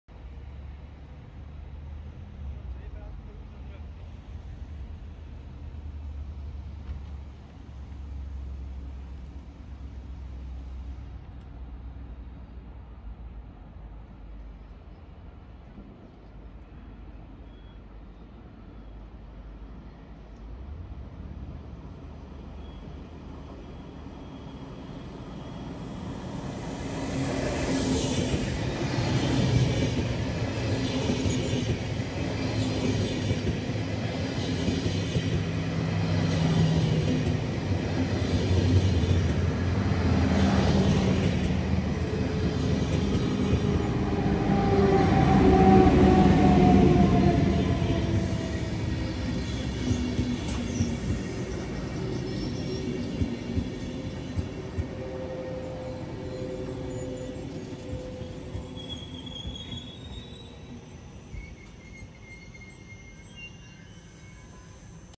Züge Rotkreuz, Bauarbeiten mit Einspurbetrieb
Am 02. und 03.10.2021 finden Bauarbeiten zwischen Rotkreuz und Ebikon statt, wobei verschiedene Weichen im Bahnhof Gisikon-Root ersetzt werden. Dadurch ist zwischen Rotkreuz und Luzern nur noch ein Einspurbetrieb möglich, mit beschränkter Kreuzungsmöglichkeit in Ebikon und diversen Fahrplanumstellungen und Zugausfällen.
IR 75 Zürich - Zug - Luzern. Dieser Zug hat ein Zusatzmodul mit 4 Wagen, was im Regelbetrieb so nicht vorkommt. Zugslänge ca 360 m